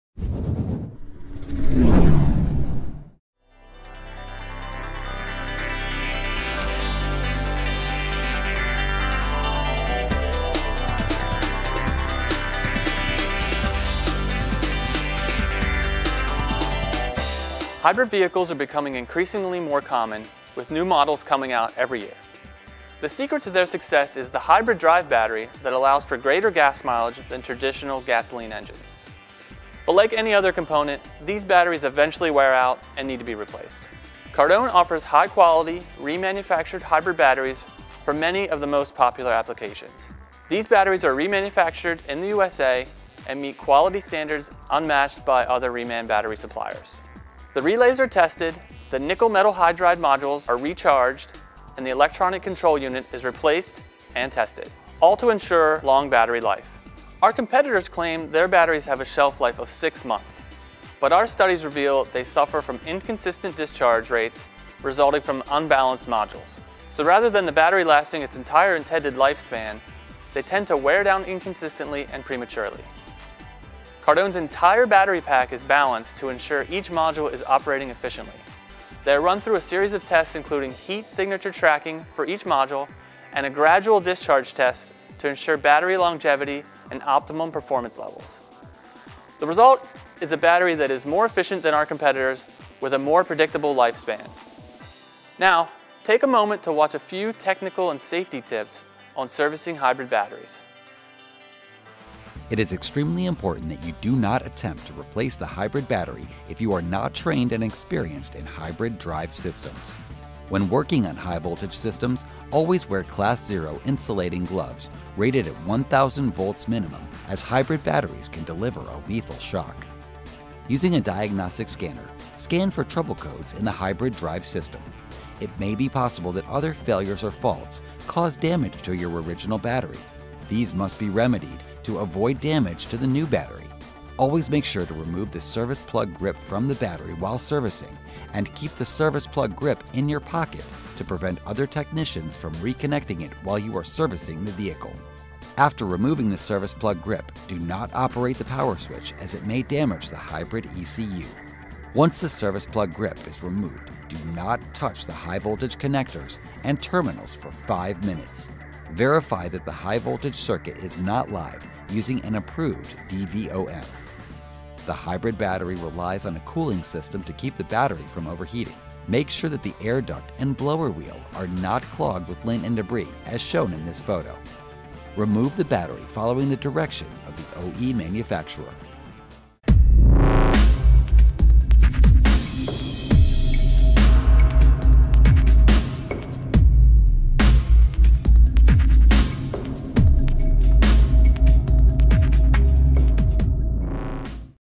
Cardone Hybrid Battery Installation Tips
in this video you will learn why Cardone remanufactured hybrid batteries outperform the competitors. Also, learn a few important tech and safety tips to remember when servicing hybrid batteries.

Tips:
- DO NOT attempt to replace the hybrid battery if you are not trained and experienced in hybrid drive systems
- Always wear class 0 insulating gloves rated at 1000 volts minimum
- Failures or faults cause damage to your original battery. These must be remedied to avoid damage to the new battery
- Always make sure to remove the service plug grip from the battery while servicing and keep the service plug grip in your pocket to prevent other technicians from reconnecting it while you are servicing the vehicle
- After removing the service plug grip, do not operate the power switch as it may damage the hybrid ECU
- Once the service plug grip is removed, DO NOT touch the high-voltage connectors and terminals for five minutes
- Verify that the high-voltage circuit is not live using an approved DVOM
- Hybrid battery relies on a cooling system to keep the battery from overheating
- Make sure that the air duct and blower wheel are not clogged with lint and debris
- Remove the battery following the direction of the OE manufacture